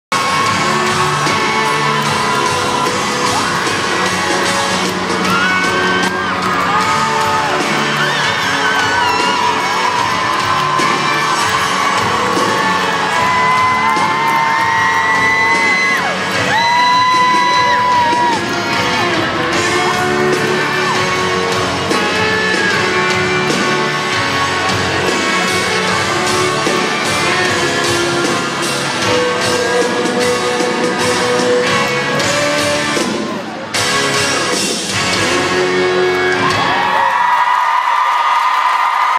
Holly & Victoria Miller's first concert
Carrie Underwood concert Baltimore- first concert ever-9 yrs old
11-7-12